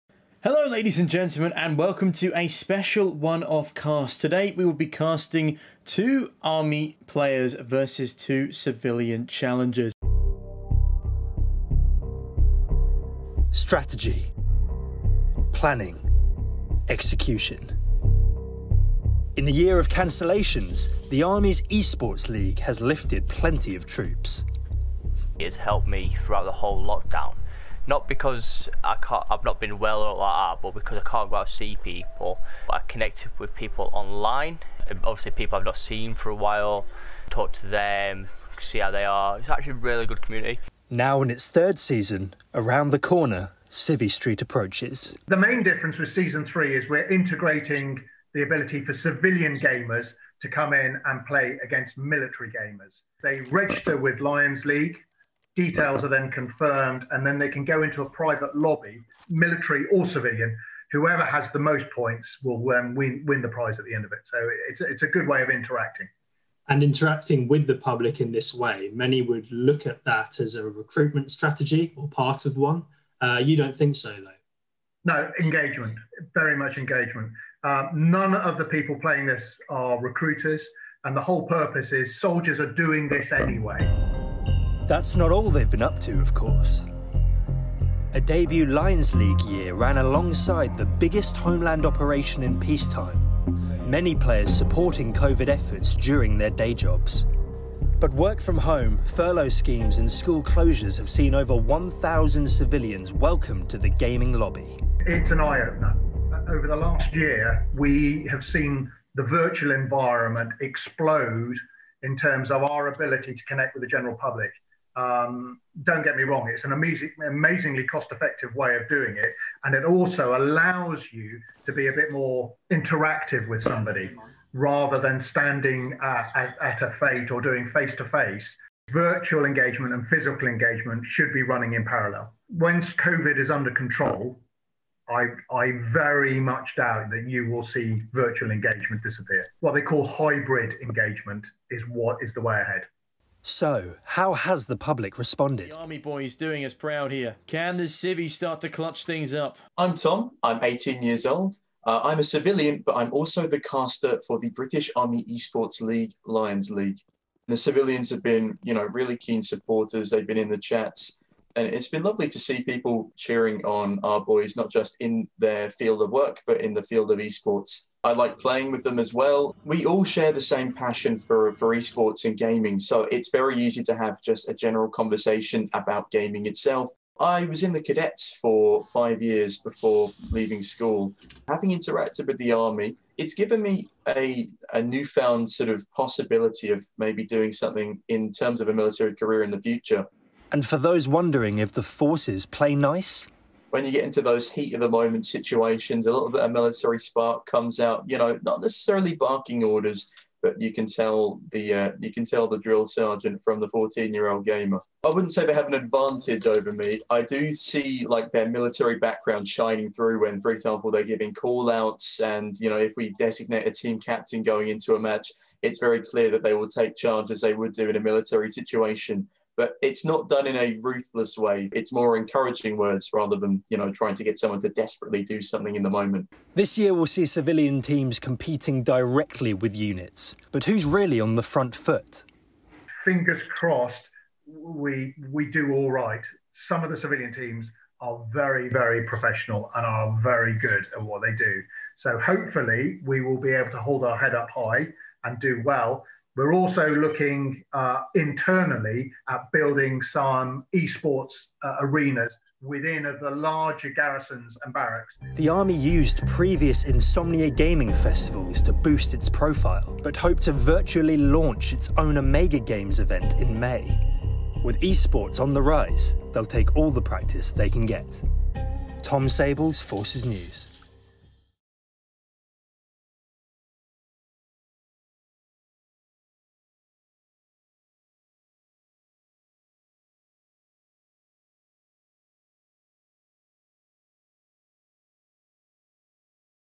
💻 🎮 e-Sports: When MILITARY TEAMS Meet CIVILIAN TEAMS
Army e-Sports is welcoming the public to its official gaming platform, which launched in March 2020.

Has the coronavirus lockdown changed engagement strategies when it comes to online gaming?